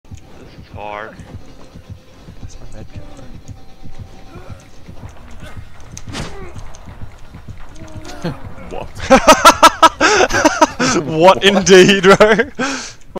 stealing back my medkit from Bubba
when Dead Hard actually works